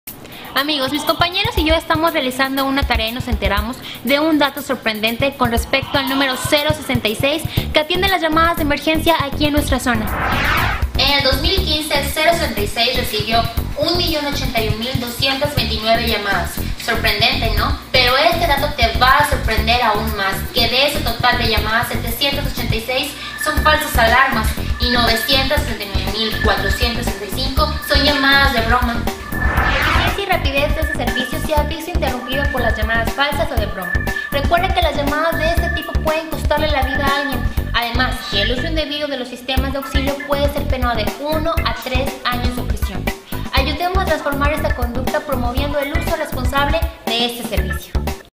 Uso responsable del 066
Te invitamos hacer uso responsable del 066. Recuerda que es para denuncias serias como: accidentes, robos, incendios, violencia intrafamiliar, etc.

"La denuncia ciudadana es tu derecho"